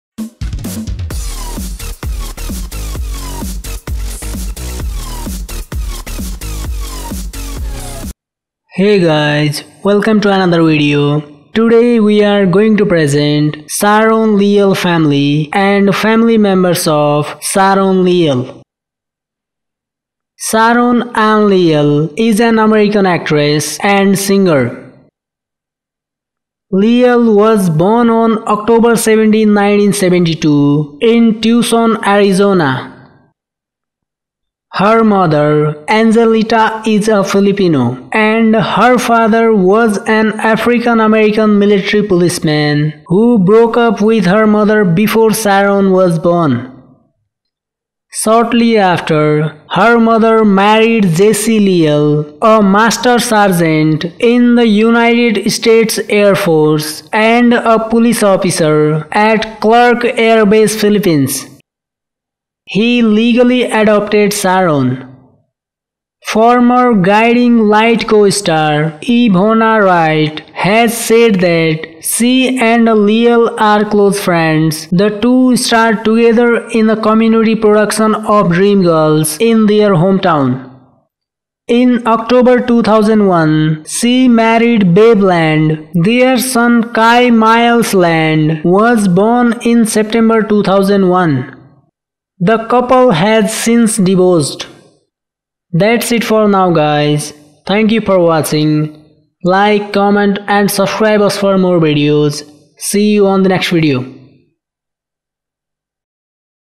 Sharon Leal Family ★ Family Of Sharon Leal
Family Of Sharon Leal and Paul Becker - Famous Celebrity and American actress Sharon Ann Leal was born in Tucson, Arizona. Her mother, Angelita, is Filipino. Her father was an African-American military policeman who broke up with her mother before Sharon was born.

Mother: Angelita Leal
Father: Jesse Leal
Former spouse: Bev Land
partner: Paul Becker
Son: Kai Miles Land
Sister: Kristina Manankil